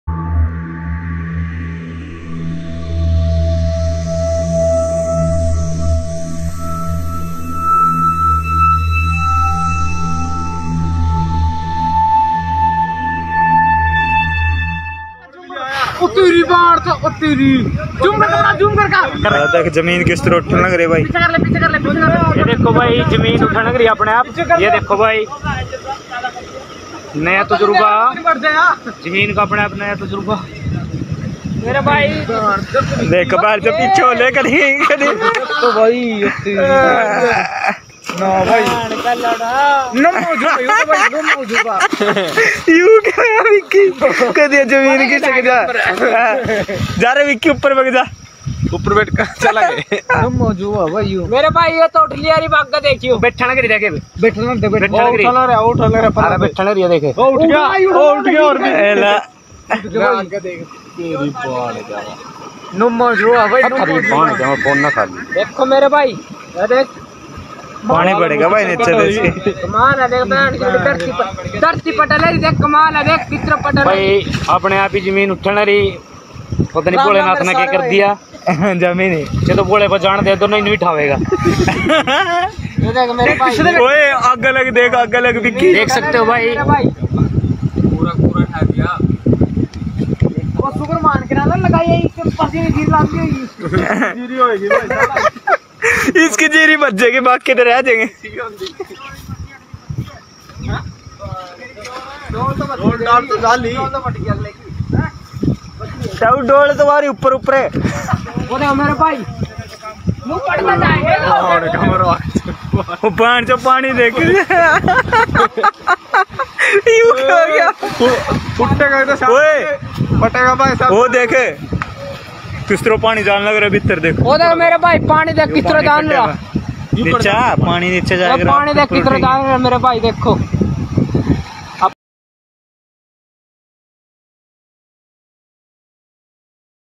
Un video muestra cómo la tierra está creciendo por sí sola en la India
En el video de casi 2 minutos, un área que está sumergida bajo el agua de repente comienza a elevarse, dejando a los presentes conmocionados. Se escucha a la persona que graba el video pidiendo a la gente que retroceda mientras la tierra continúa elevándose.

“Esta es una nueva experiencia”, se puede escuchar a los hombres en el video mientras muestran la vasta área donde el suelo se ha elevado. “Esto es increíble. La tierra está creciendo por sí sola. No tenemos idea de lo que está sucediendo.”

Hasta el momento nadie ha podido ofrecer una explicación científica al misterioso fenómeno, por lo que los internautas ofrecieron sus propias teorías. Mientras que algunos dijeron que la tierra se elevó debido a la actividad tectónica, otros debatieron que se debía al metano atrapado en la tierra.

“Esto sucedió debido a actividades tectónicas. Es muy peligroso quedarse aquí. Quizás ocurrió algo de actividad volcánica”, escribió un usuario, mientras que otro comentó: “No realmente debido a la actividad tectónica, pero el metano atrapado en la tierra se libera y la capa húmeda forma una burbuja, eso es lo que parece estar sucediendo aquí.”